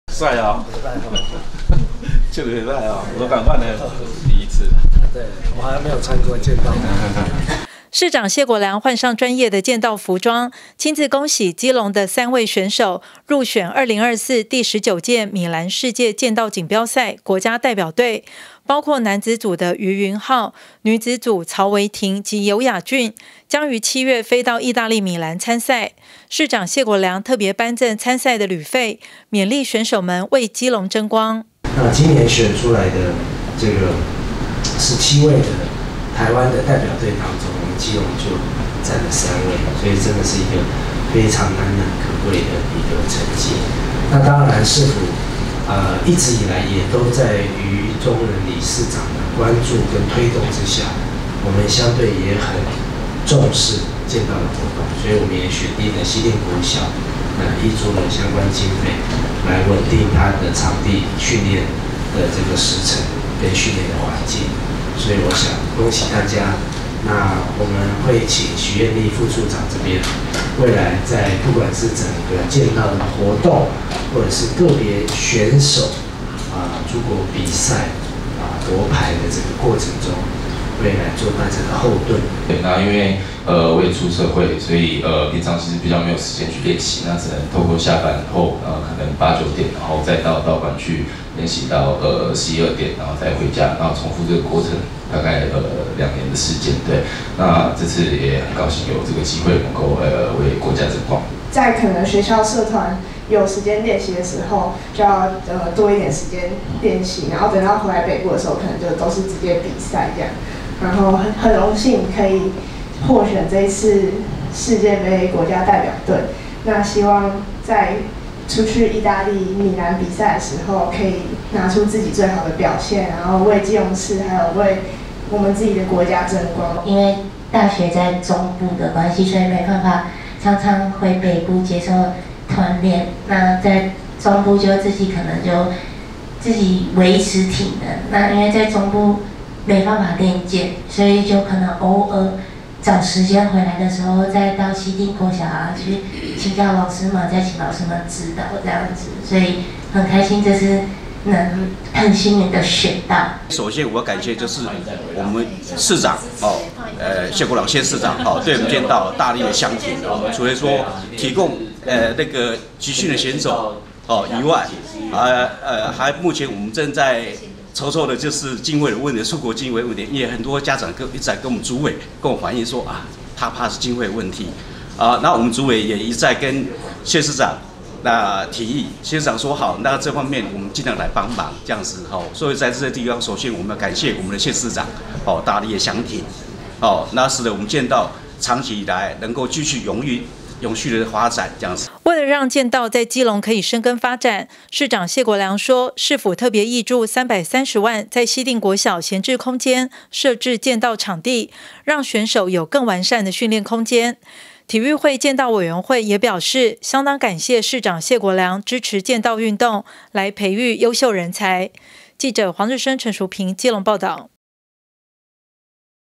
基隆之光! 3選手七月出征世界劍道錦標賽 謝國樑勉勵
三年一次的「2024第十九屆米蘭世界劍道錦標賽」將於今年在義大利米蘭盛大舉行，各國都將派好手應戰。我國日前完成國家代表隊選拔，基隆市有3位選手獲青睞，順利拿到「國家代表隊資格」，市長謝國樑換上專業劍道服裝，親自接見選手並頒贈參賽旅費，為選手加油打氣！

基隆市體育會劍道委員會主委余宗仁指出，世界劍道錦標賽(World Kendo Championship，WKC)是由國際劍道聯盟主辦的國際賽事。今年很榮幸基隆市有3位在地選手入選國家隊，包括男子組的余昀浩、女子組曹維婷及游雅珺，將於今年七月飛到義大利米蘭參賽。

市長謝國樑親自恭賀獲選為國手們的基隆學子，表示為國爭光的訓練過程必定非常考驗身心靈，期許大家莫忘初心，發揮自己的最大值！同時，謝國樑也提及為使劍道在基隆可以深耕發展，市府特別挹注330萬在西定國小的閒置空間設置劍道場地，讓更多選手有安全良好的訓練空間。

此次獲選男子組國手的余昀浩表示，身為上班族的他，利用平日下班後及假日時間，每晚都在訓練場練習至半夜，對於此次比賽有著偌大的期待，期許自己好好發揮，為國爭光！女子組國手游雅珺提到自己目前就讀高雄醫學大學，備賽時期南北往來通勤，耗時且耗財等問題都要一一克服，但今日入選國家代表隊，很感謝身邊家人朋友的支持，讓她有毅力繼續訓練，完成目標。另外，年僅大一的曹維婷表示，可以代表台灣出國比賽真的非常興奮，從小學開始學習劍道，一直期待有機會可以參與國際賽事，這次將代表台灣出征劍道運動世界殿堂，她一定會更扎實的準備比賽，珍惜出國比賽的機會！